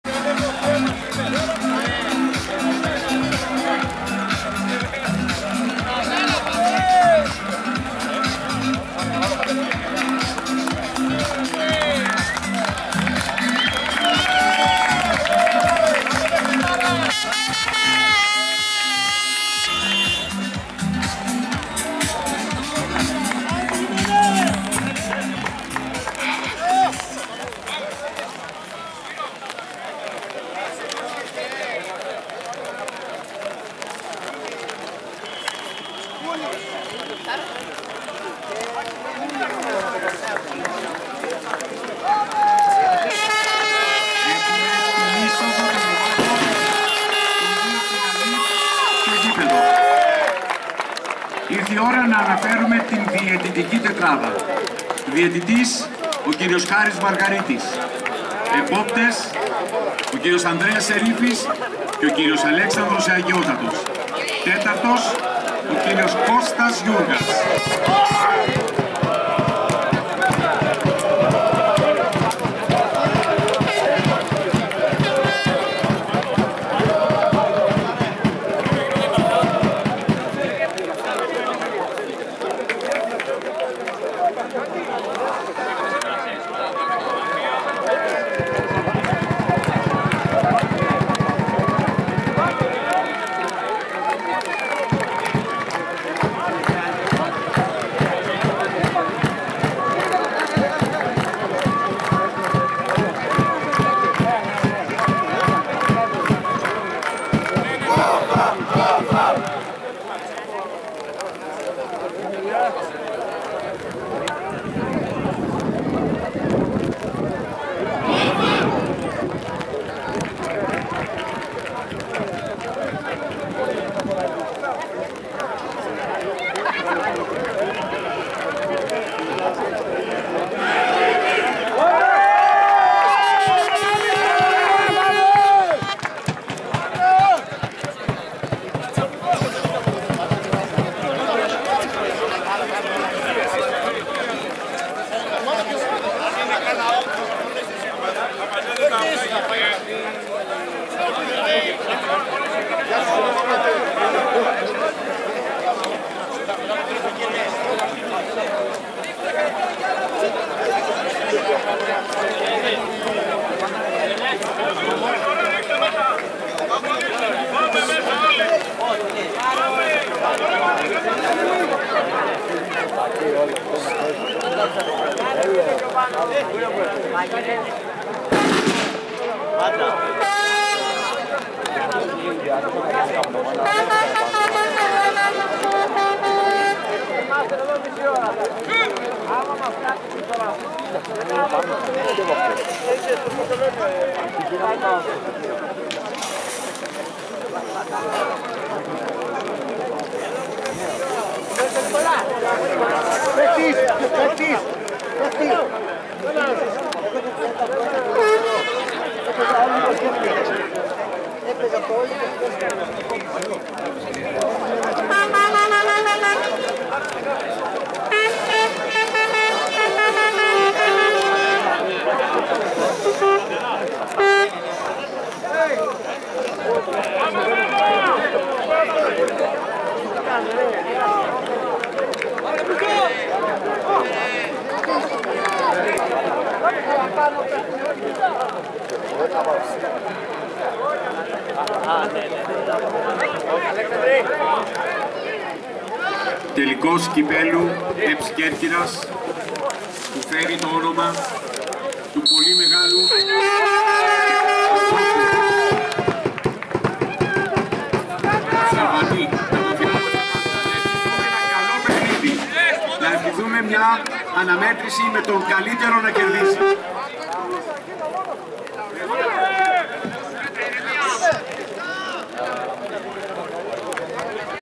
Τελικός Κυπέλλου Ε.Π.Σ. Corfu,Α.Ε Λευκίμμη-Ο.Φ.Α.Μ,Είσοδος Ομάδων-Παράταξη -Χαιρετ.-Κερκίδα-Έναρξη
Η Εκδήλωση του τίτλου στο γήοεδο του Ολύμπου την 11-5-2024 και η είσοδος των Ομάδων  στον αγωνιστικό χώρο & έως και 15 μετά την έναρξη με σιγανή βροχή.!Γιαυτό και η λήψη αυτού από την δυτική πλευρά του αγωνιστικού χώρου(προς Κερκίδα')με ομπρέλα και με το ένα χέρι για να ..προστατευθεί η κάμερα.!Γιαυτό και η μή επαρκής σταθερότητά του.!Α.Ε. Λευκίμμη(με λευκές εμφανίσεις) -Ο.Φ.Α.Μ(Αγίου Ματθαίου).!Σε αυτό η είσοδος των Ομάδων στον αγωνιστικό χώρο σε παράταξη-Χαιρετισμοί,Πάγκοι Ομάδων-ΚερκΊδα,έναρξη του  αγώνα ,οι Διααιτητές ακούστηκαν από το Εκφωνητή του Τελικού ,τον Δημοσιογράφο Νίκο Άνθη.! Corfu Union Champoinship for local Cup,between the Teams F.C. Lefkimmi-F.C. O.F.A.M.,i Olgmpos F.C.Football-Ground, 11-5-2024.!With rain until the 15 minute after the start of it.!Recieving of this  video under Umbrella & one Hand Hold of Camer,that's why not very stable.!All the  start procedure  before the Game here  & until some moments of the Game.!.!